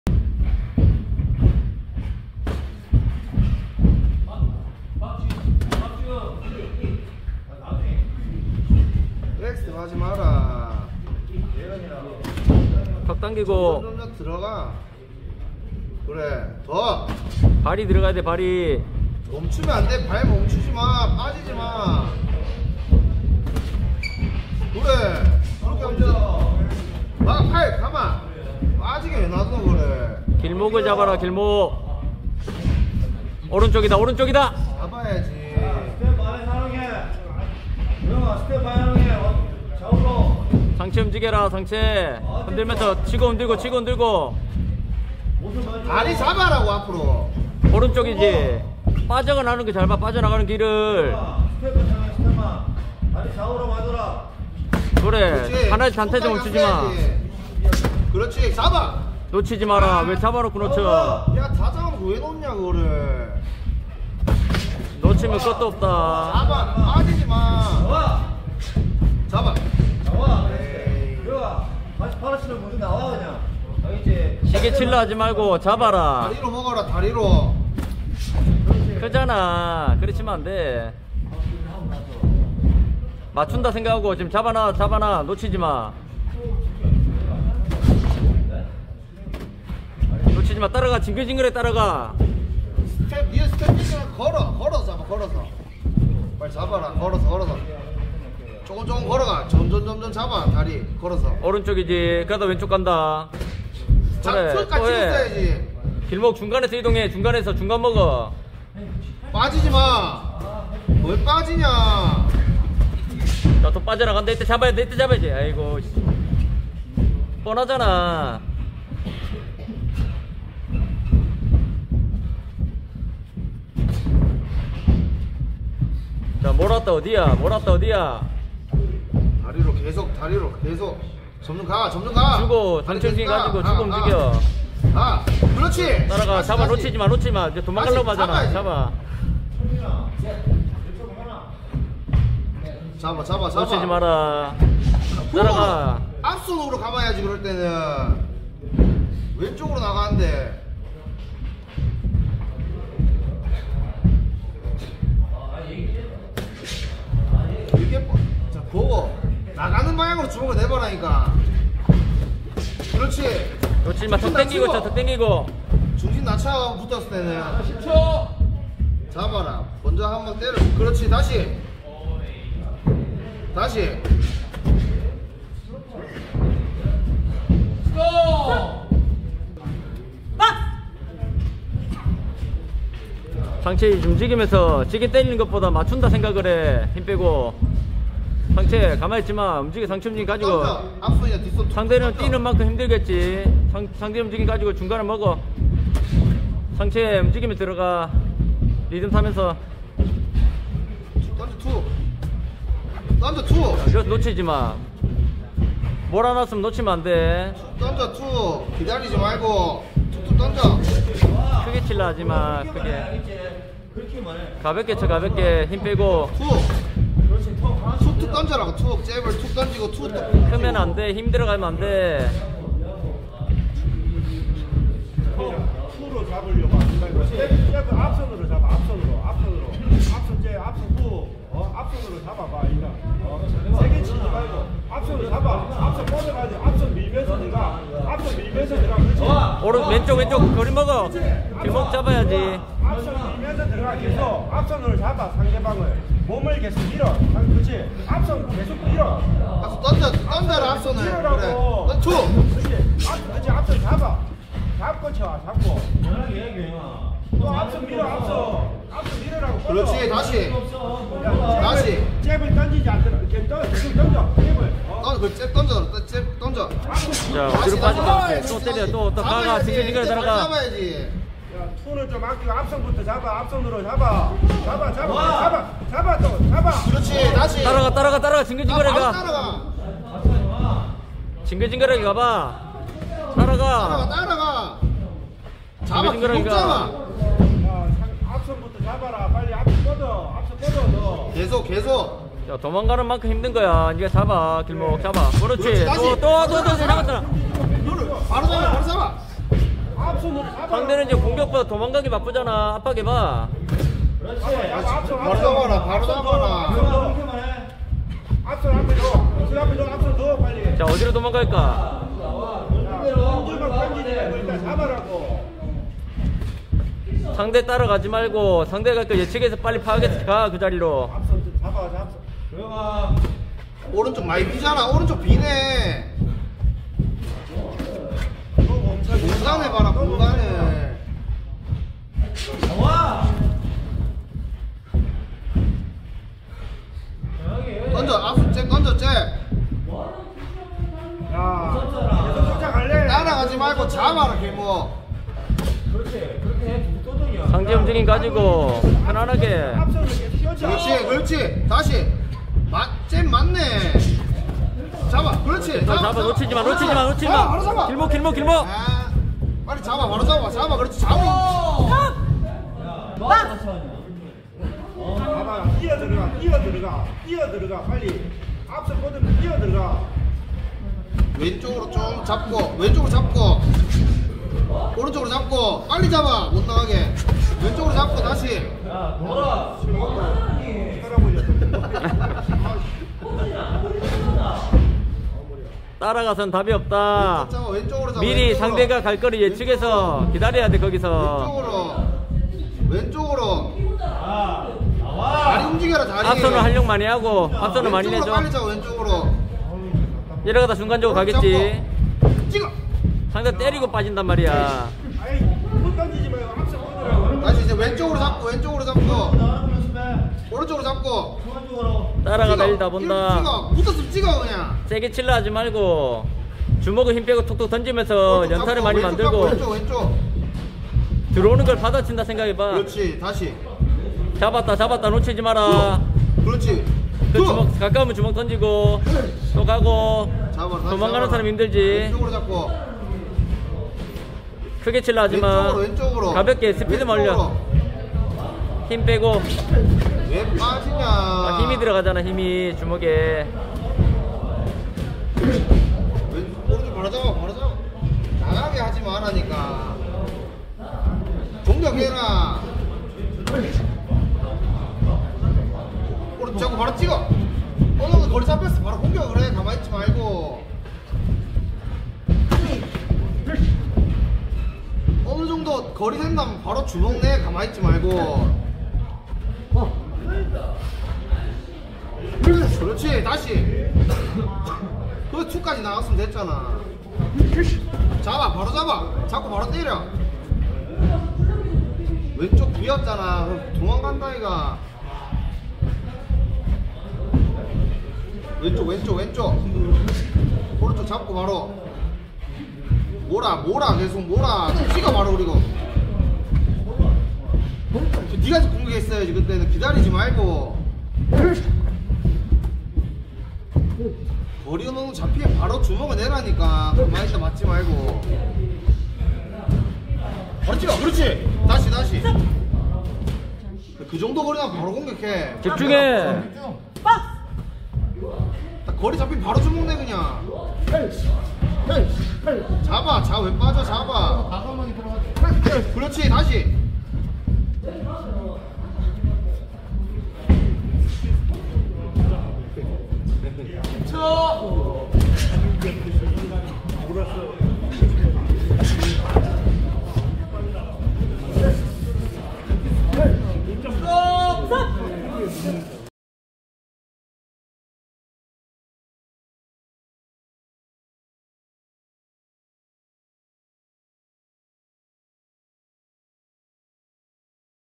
신인 프로복서 VS 아마복서 에이스 찐 스파링!! 거리를 잡아야만한다..👀
스파링 합동 훈련 영상입니다.
프로복서VS아마복서